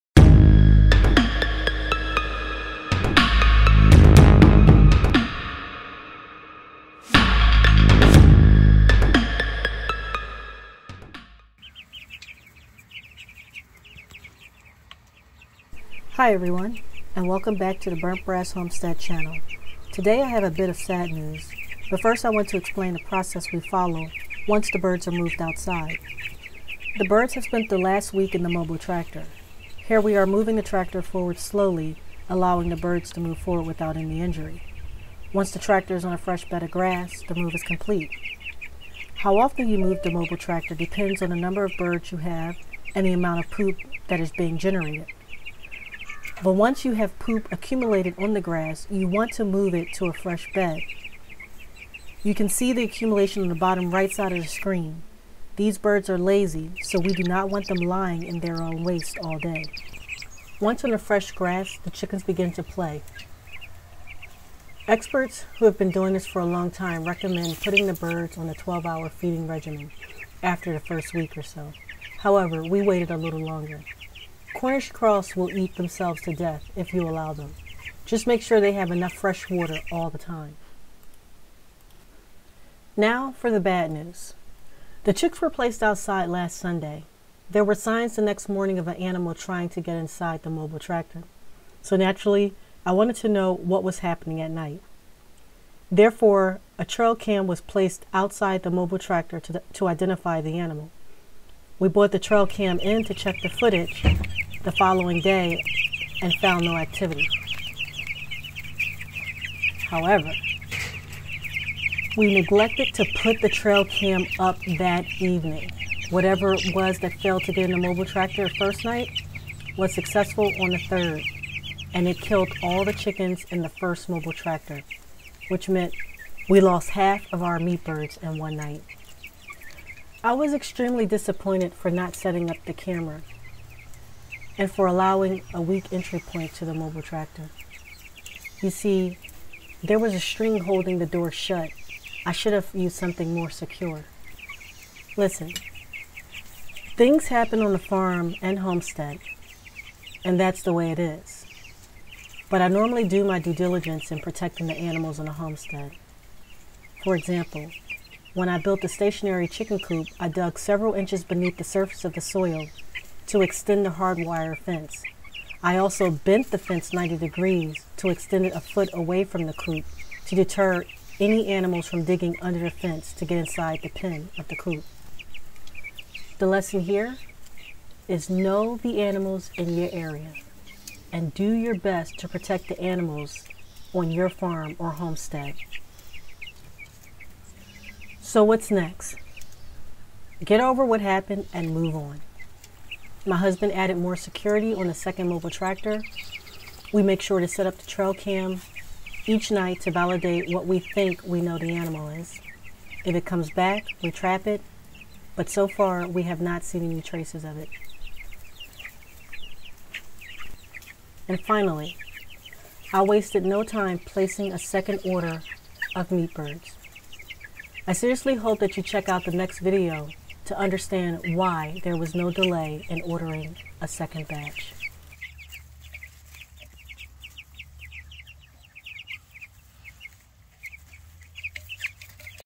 Raising Meat Chickens: Week 3 (Sad News)
This is a video update on the meat birds that arrived 3 weeks ago. This will be an 8 week series until the birds are ready to be culled.